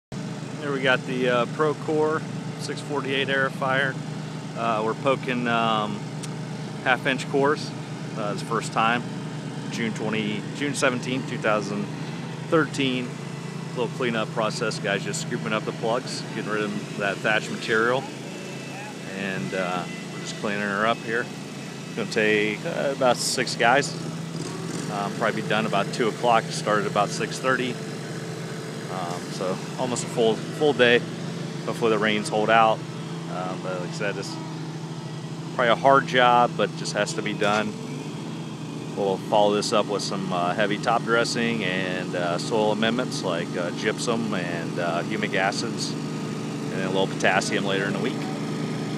Aerifying greens Vero Beach CC
Aerification on greens pro core